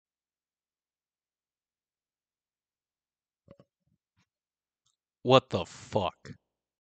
2022 in 6 seconds
New friends, and 2 losses later, I'd say 2022 was good for my channel. As for me personally, not so much. But let's not worry about that right now! Happy new year everyone! And yes, I shaved.